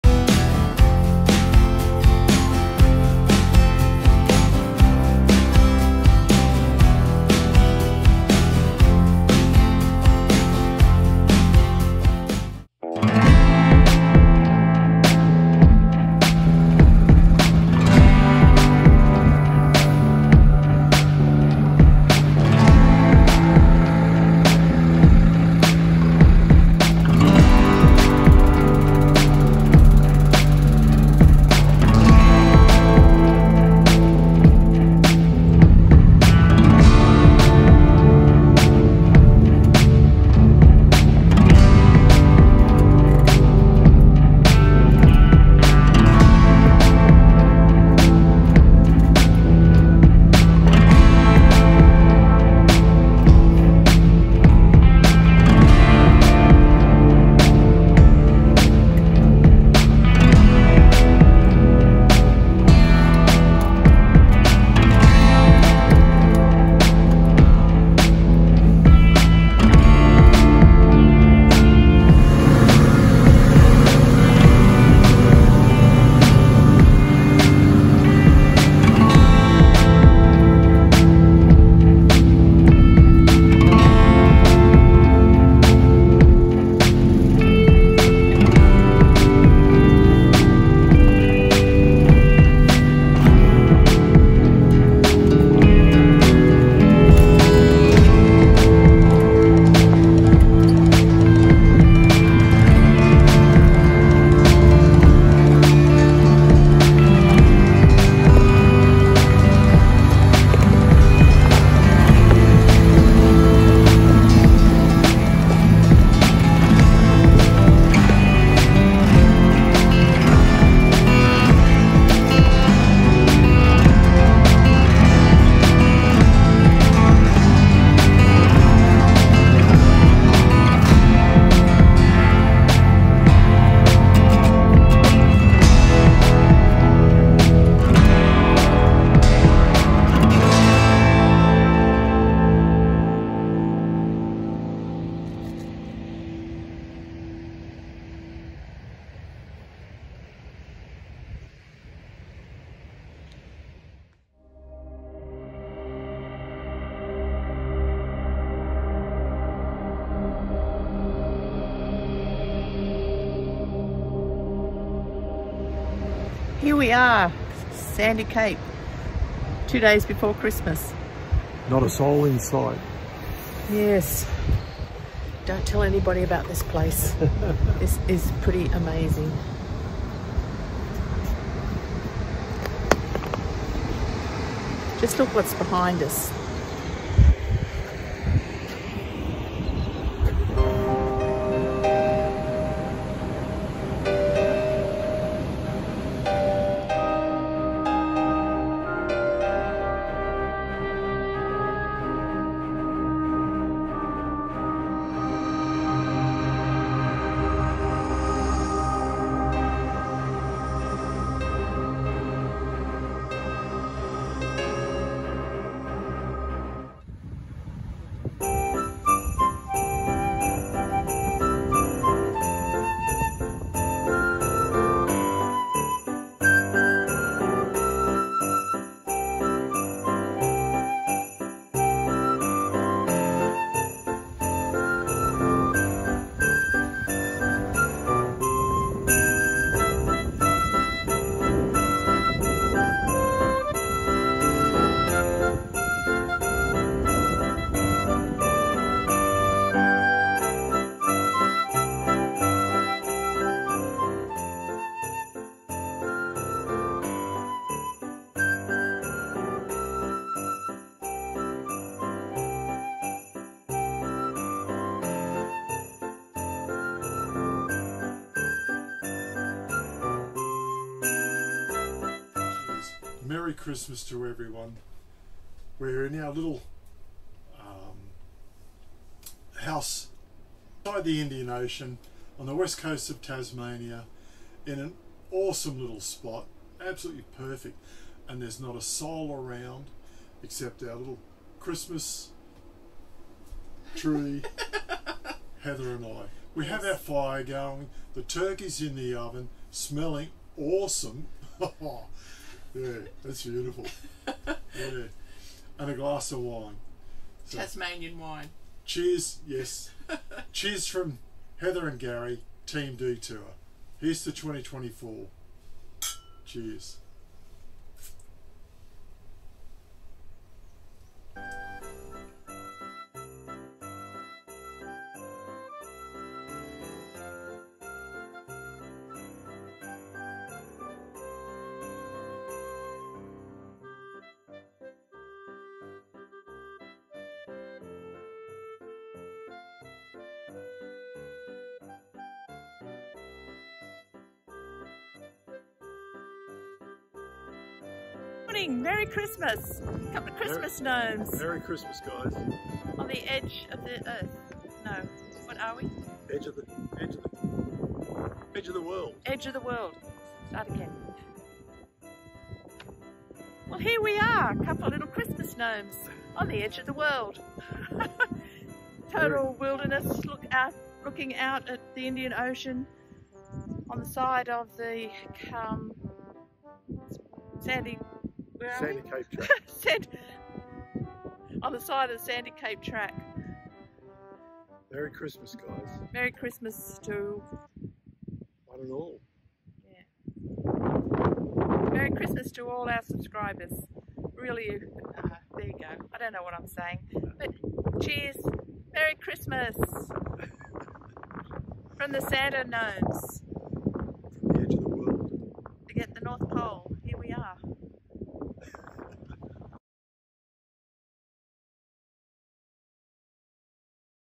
Unbelievable Christmas Escapade in Tasmania!
Join us on an incredible journey as we explore the stunning landscapes of Tasmania during our unforgettable Christmas escapade! In this video, we venture through the breathtaking Sandy Cape Track, where nature's beauty collides with the festive spirit. Experience the unique charm of a Tasmanian Christmas as we uncover hidden gems, local traditions, and picturesque views that will leave you in awe. From scenic walks to festive celebrations, this adventure is packed with unforgettable moments. Don't miss out on this magical holiday experience in one of Australia's most captivating regions! Tune in and let the adventure begin!